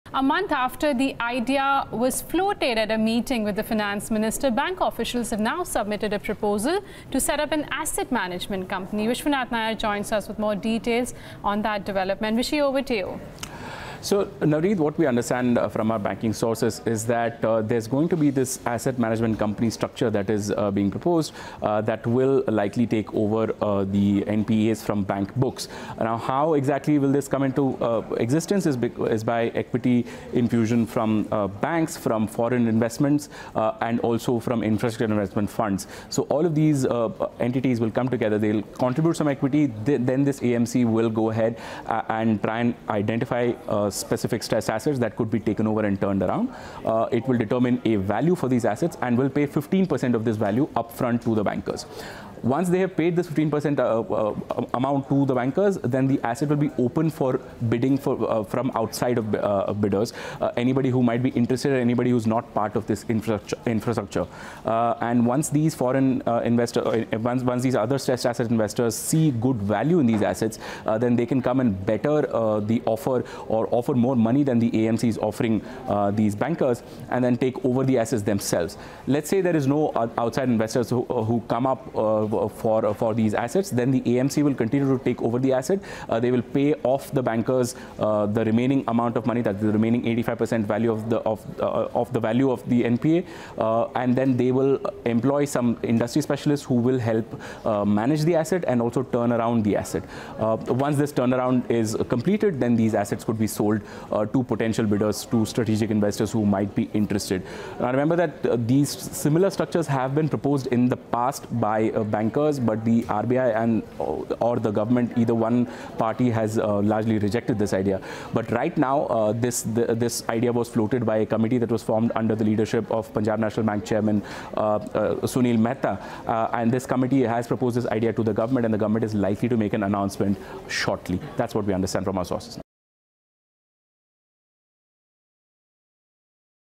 BQ Exclusive | Banks Draw Up A Plan For AMC
Banks propose a joint AMC to take over bad loans.